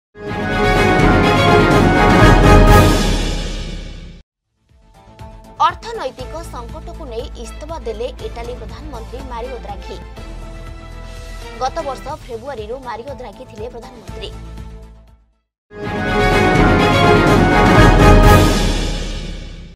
Italy prime minister Mario Draghi steps down | Kalinga Scroll
Italian government on the brink after PM offers to resign.